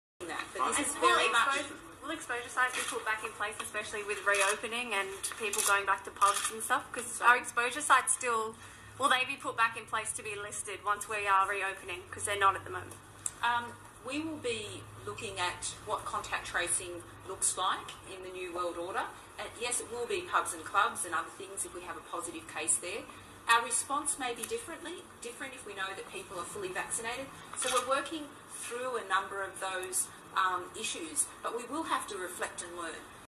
Here Comes the Flying Monkeys: We'll be looking at contact tracing looks like in the New World Order
Here Comes the Flying Monkeys! Australian health chief says "We will be looking at contact tracing looks like in the New World Order."

Why is Australia shutting down internet?

James Jesu-White LOVES The Tech Behind The Covid Vaccines!

NO VAX FOR GOOGLE EMPLOYEES, POLITICIAN OR ANY WORLD LEADER-ONLY SHEEP QUALIFY FOR THE JAB- MIRRORED